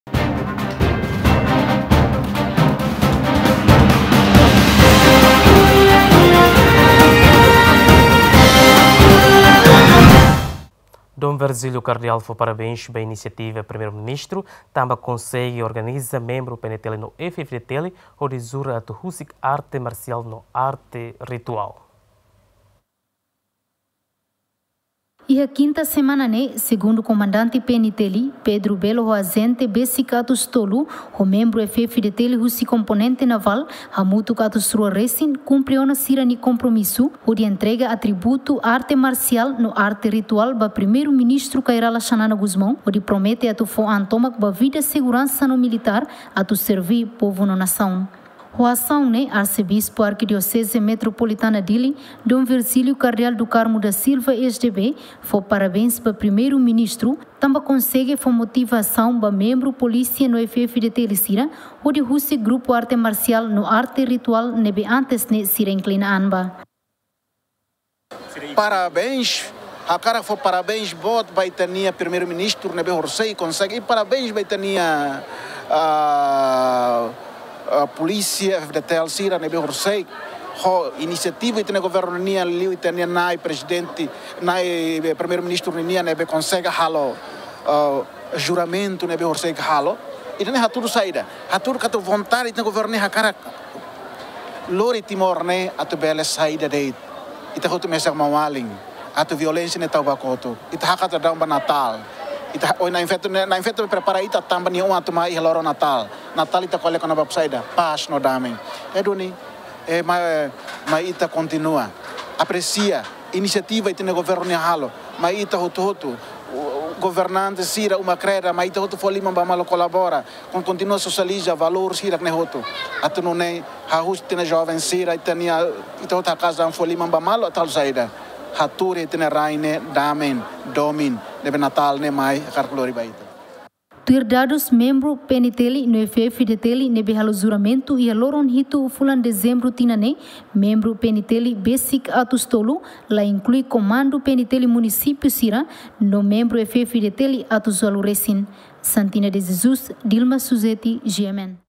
Kardeal fó parabens ba PM tanba konsege organiza membru PNTL no F-FDTL hodi jura atu husik AM no AR
Kardeal fó parabens ba inisiativa Primeiru Ministru tanba konsege organiza membru PNTL no F-FDTL hodi jura atu husik arte marsial no arte ritual